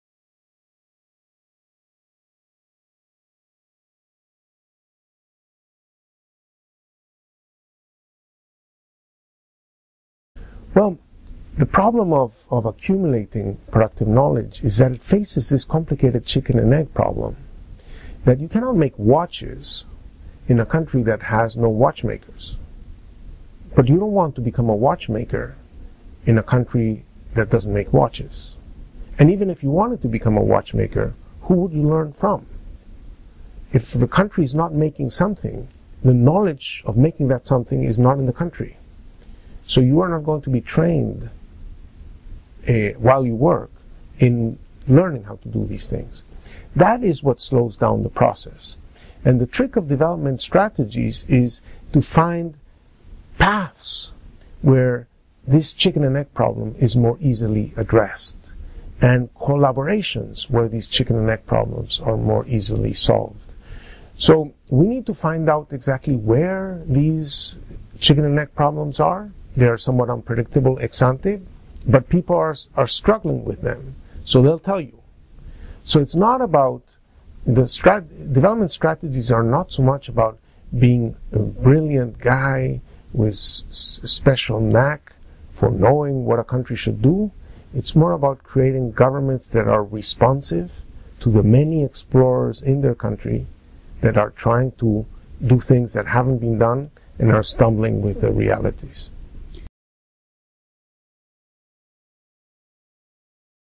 Ricardo Hausmann (1/3): How to Build Larger Teams
Ricardo Hausmann
Director of the Center for International Development, Harvard University

Hausmann discusses how to solve the chicken-or-egg problem of developing new economic skill sets in markets without an existing industry to train new workers.

He says the key to accumulating productive knowledge in economies is finding paths to solving this problem through collaborating with other markets and economies.

To Hausmann, Development strategies are not about being brilliant policymakers with all the answers, they are more about being responsive when needs arise in their countries and flexible in finding other economies to learn from.